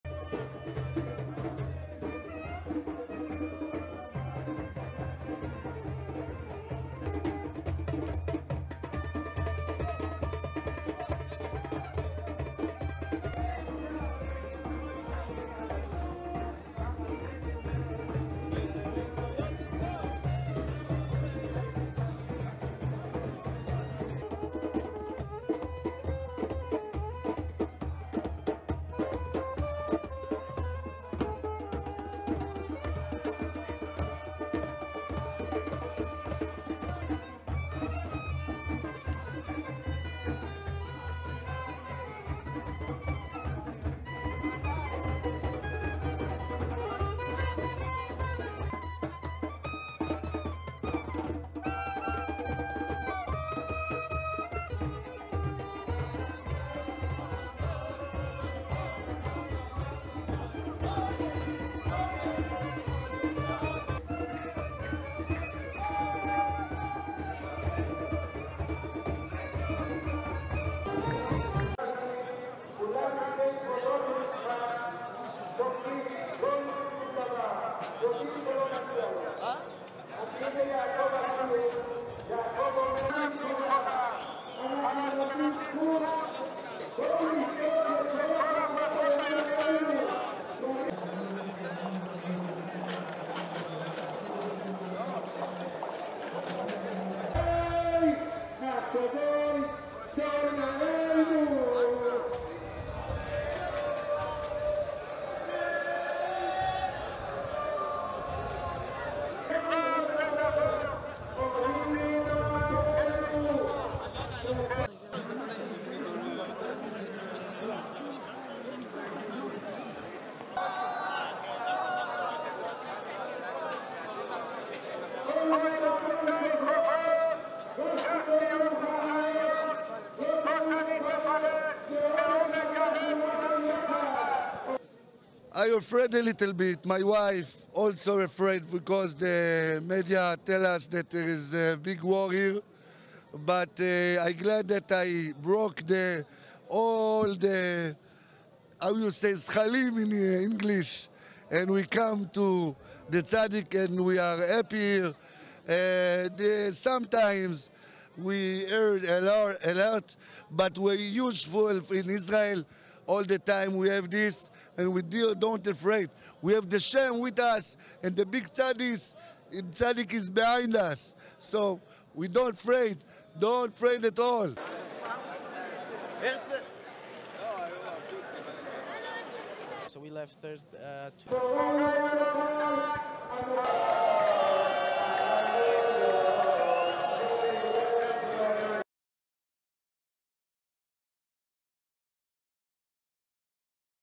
В Умані десятки тисяч хасидів святкують Рош ха-Шана | OBOZREVATEL TV
Всупереч війні та закликам не приїжджати до Умані через загрозу російських ракетних ударів, до міста приїхало понад 20 тис. хасидів.
Щодоку до Умані на єврейський Новий рік з усього світу приїжджають паломники-хасиди, щоб помолитися на могилі засновника брацлавського хасидизму раббі (цадика) Нахмана.
Святкування Рош ха-Шана цього року розпочались увечері 25 вересня та закінчаться 27 вересня

• Команда "Obozrevatel" перебуває на інформаційній передовій – разом з українським народом та армією 24/7. Ваша підтримка команді добровільними внесками допоможе продовжувати створювати для вас якісний контент
Дякуємо!